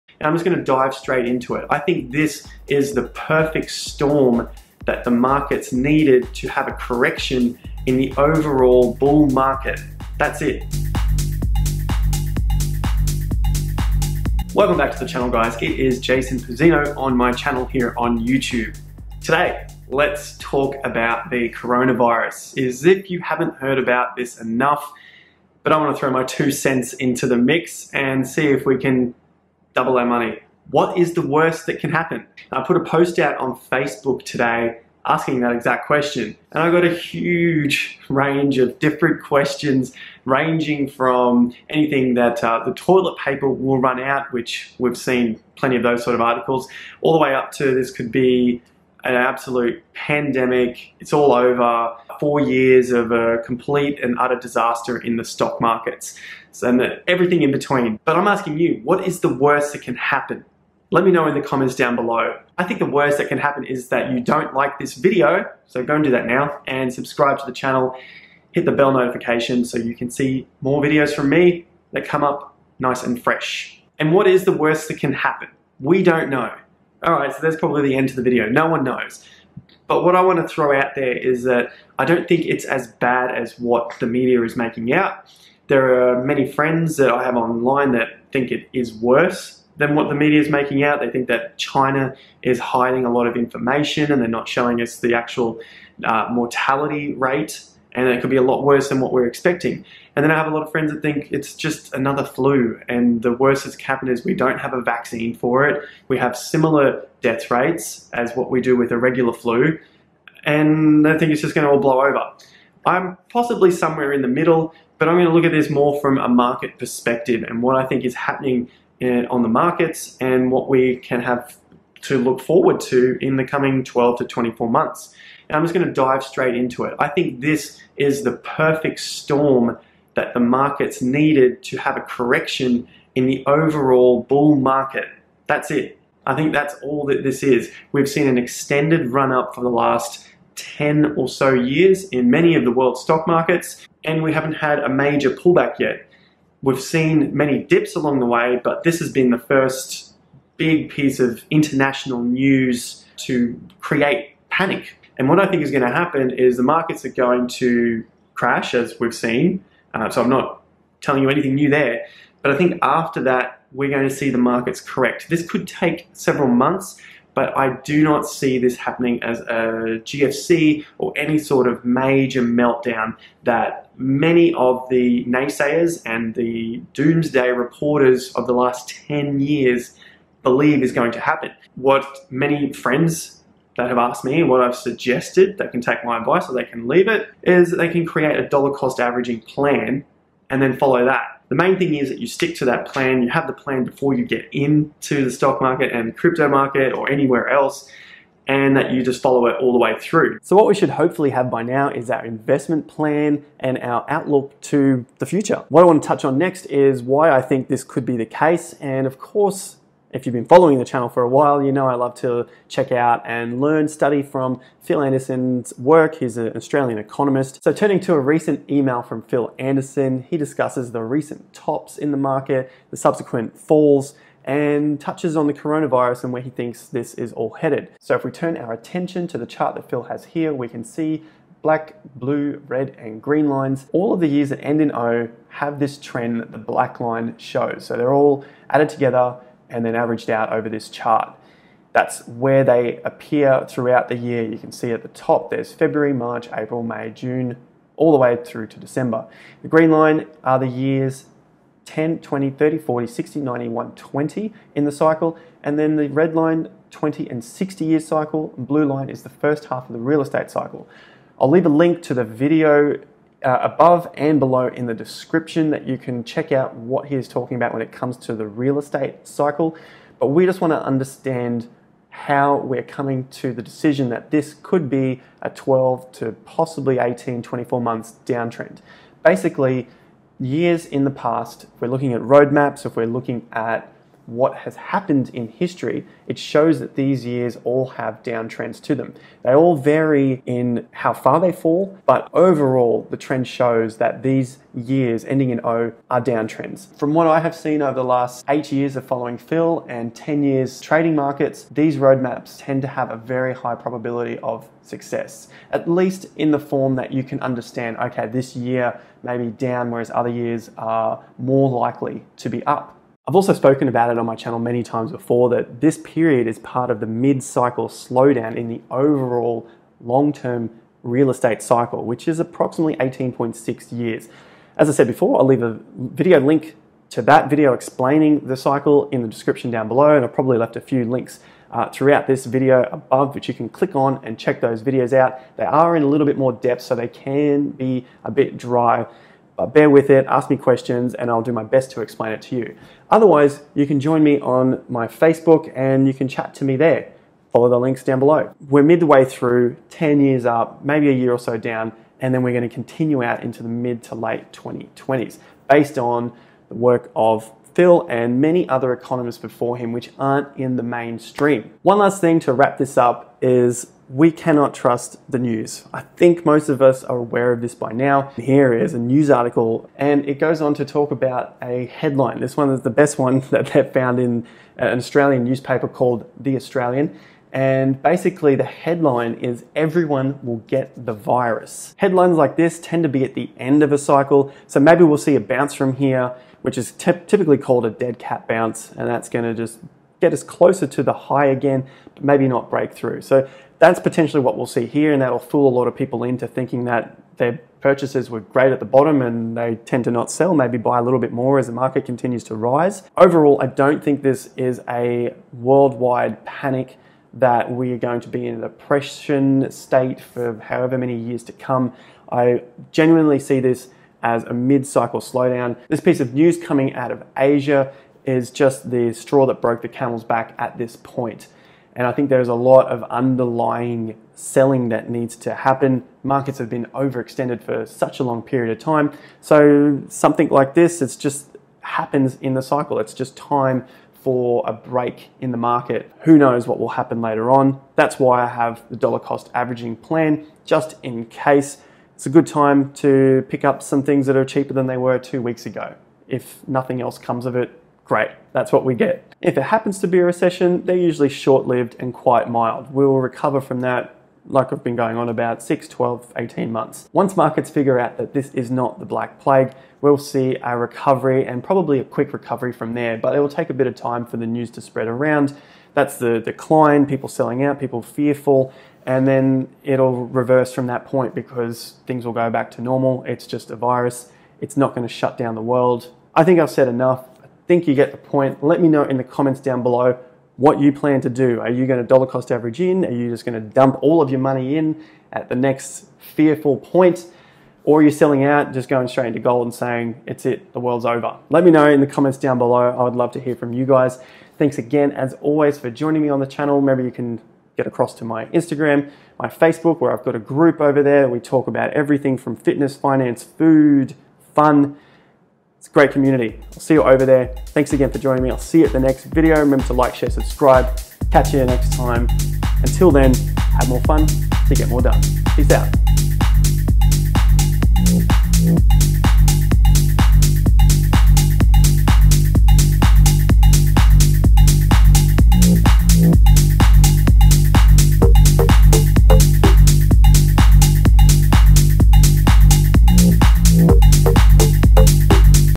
Coronavirus: WORST that can happen? SIMPLE INVESTING STRATEGY 😰📉
This video directly discusses the (unnamed) world health crisis, it's possible impact on world markets and how I am proceeding to navigate around it all.

I think most people are so sick of hearing about it. So let's keep this one brief and just discuss what we can do to make the most out of a bad situation. And I don't mean hoard all of the toilet paper! I'm talking about a simple plan to profit from the stock market, cryptocurrencies and other markets that may be affected by this international news.

In this video, I'll discuss what I think will happen based off of economic cycles which I have been following for over 8 years. These cycles have been around for hundreds of years; I just happened to learn about them, use them, and now I'm eager to share them with anyone who is keen to make money in the markets from them!

Until next time... Have more fun! Get more done!
Jason :)
FREE Cash & Cashback Links from Services I Use:
💰Stay with Airbnb for AU$76 OFF!
💰Host on Airbnb for extra cashback!